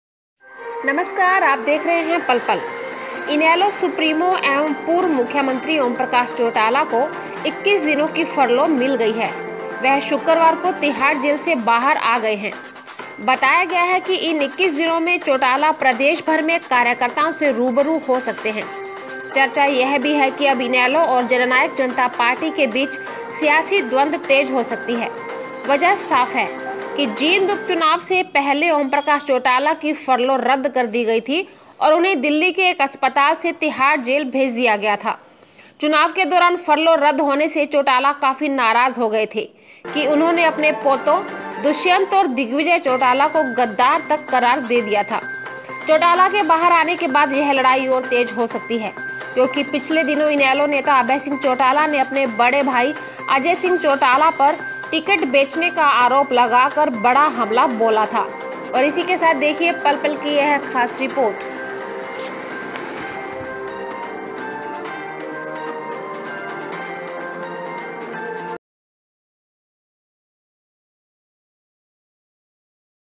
इनेलो सुप्रीमो ओमप्रकाश चौटाला 21 दिन की फरलो पर जेल से आए बहार
इनेलो सुप्रीमो ओमप्रकाश चौटाला 21 दिन की पैरोल पर जेल से आए बहार